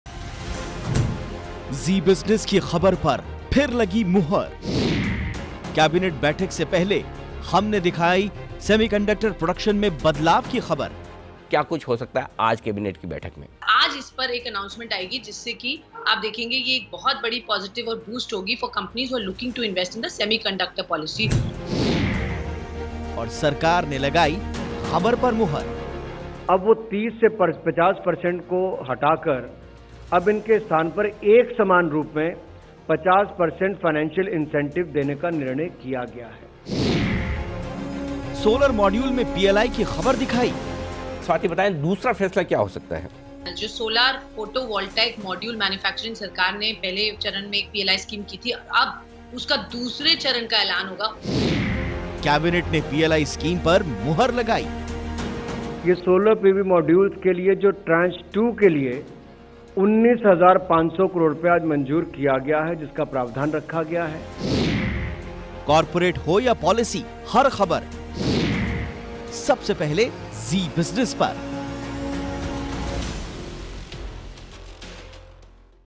Zee Business की खबर पर मुहर|Change in semiconductor scheme and 2nd Solar PV module PLI gets approval
💫ZeeBusiness की खबर पर मुहर

📜सेमीकंडक्टर स्कीम में बदलाव को मंजूरी

🌞सोलर PV मॉड्यूल के लिए दूसरी PLI के लिए ₹19,500 Cr की मंजूरी

Zee Business ने पहले ही दिखाई थी ये खबर